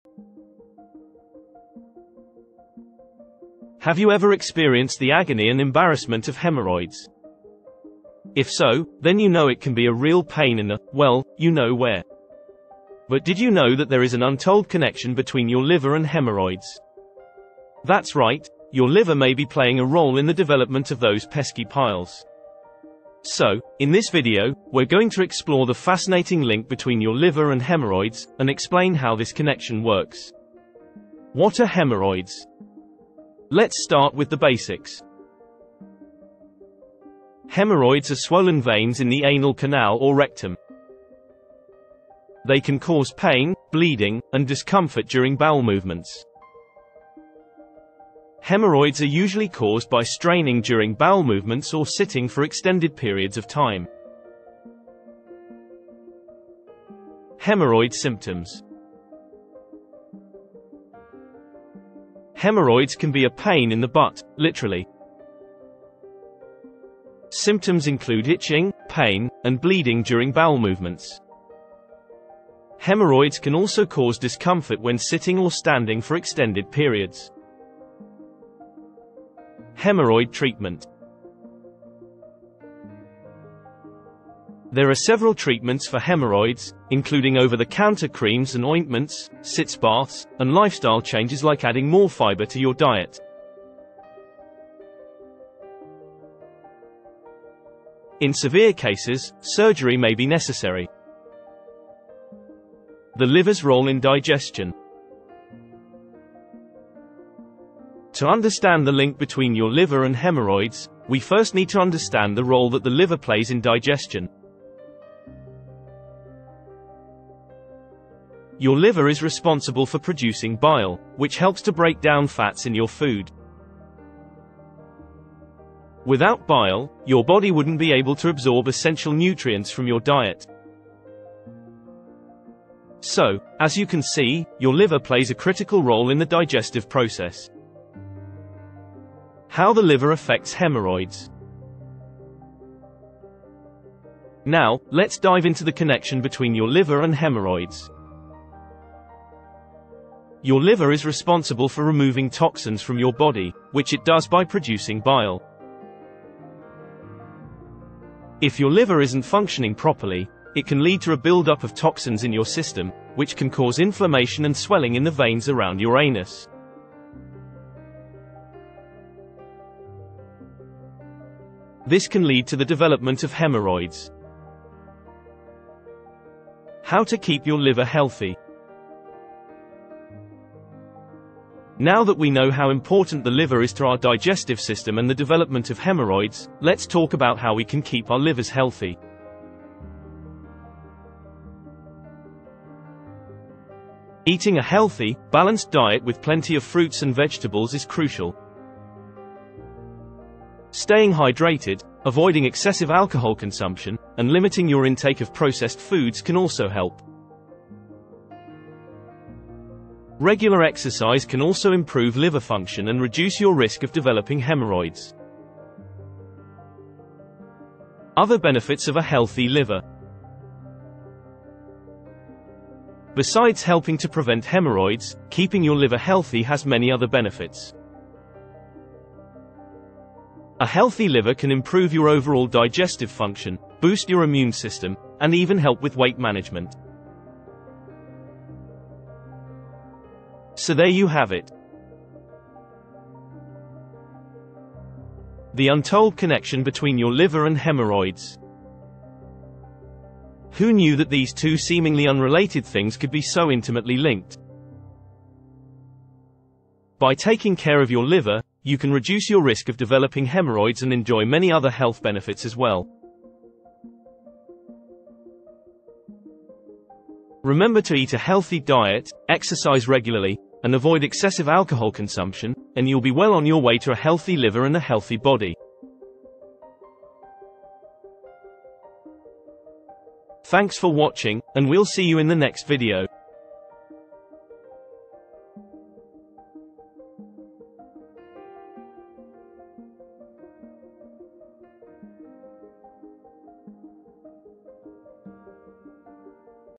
Understanding the Connection Between Liver and Hemorrhoids 👍liver
Gain a professional understanding of the connection between liver and hemorrhoids with this video. Get the insight and knowledge you need to take control of your health and make informed decisions about your health.

Welcome to Health After 50 and a video that will change the way you think about your health! In this informative and eye-opening video, we will delve into the untold connection between your liver and hemorrhoids. You might be thinking, "What does my liver have to do with hemorrhoids?" Well, you'll be surprised to know that your liver plays a critical role in your body's digestive system, and a malfunctioning liver can lead to various health complications, including hemorrhoids.

We will explore the science behind this link and discuss the various symptoms and warning signs you should look out for. But don't worry, we won't just leave you with a problem. We will also provide you with practical tips and strategies to improve your liver health, which will not only reduce your chances of getting hemorrhoids but also boost your overall well-being.

So sit back, relax, and get ready to learn about the untold connection between your liver and hemorrhoids. We guarantee that you'll come away from this video feeling more informed and empowered to take charge of your health. Let's get started!

(062)